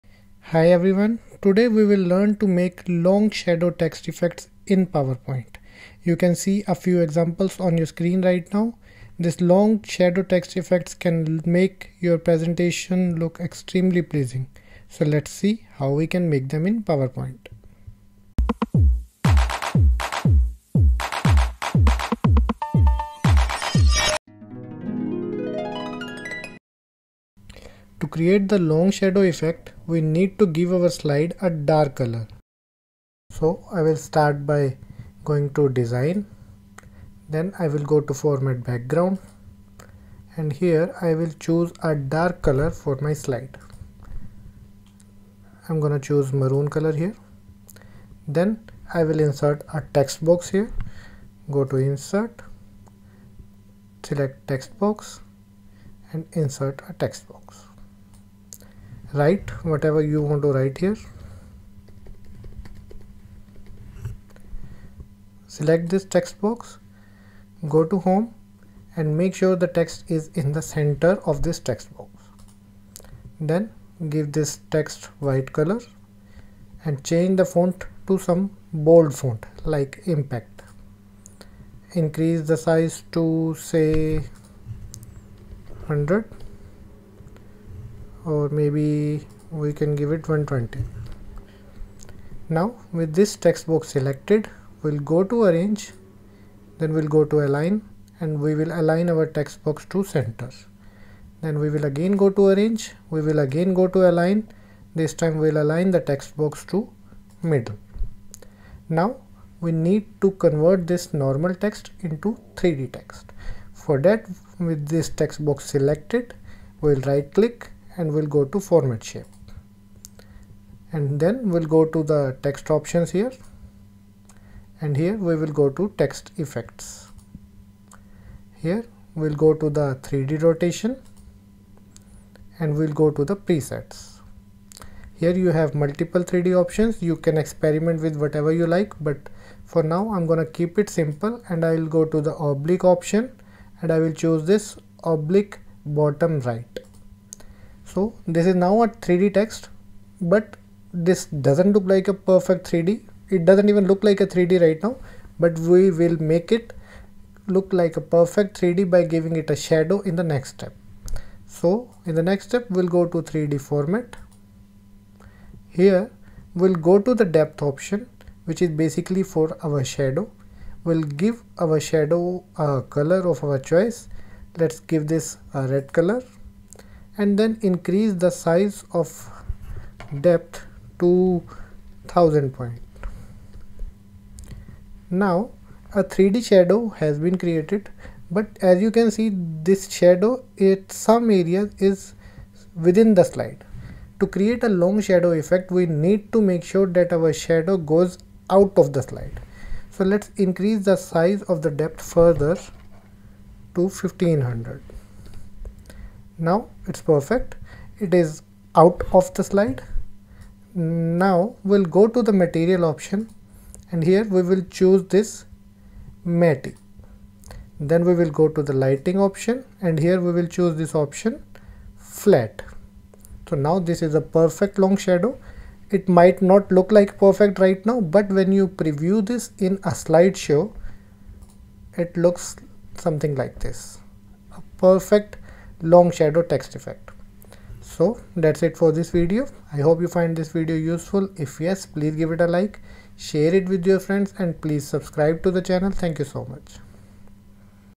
Long Shadow Text Effect in PowerPoint
In this PowerPoint tutorial, we'll demonstrate how you can create long shadow text effect in PowerPoint.
Long Shadow Text Effect can be really useful to make pleasing and eye-catchy presentations in PowerPoint.

With this tutorial, you'll also go through under-mentioned basics of PowerPoint:
1. How to create 3D Text in PowerPoint
2. How to use text effects in PowerPoint.
3. How to format background in PowerPoint.